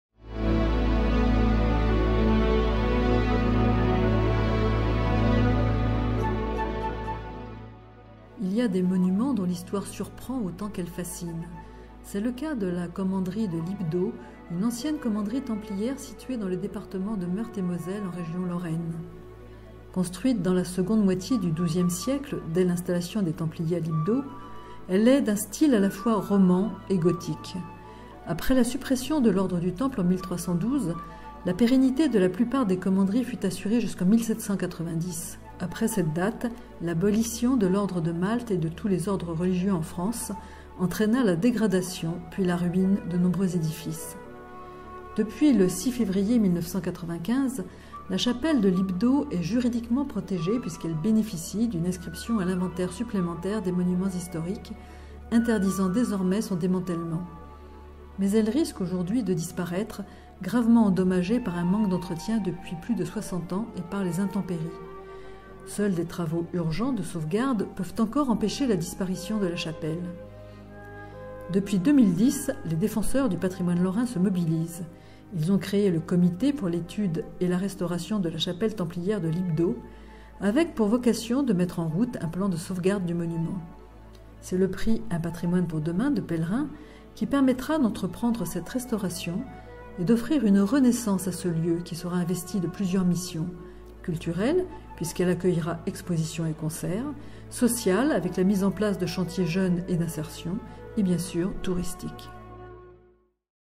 Chapelle de la commanderie des Templiers de Libdeau à Toul
Lauréat du concours Un patrimoine pour demain 2013.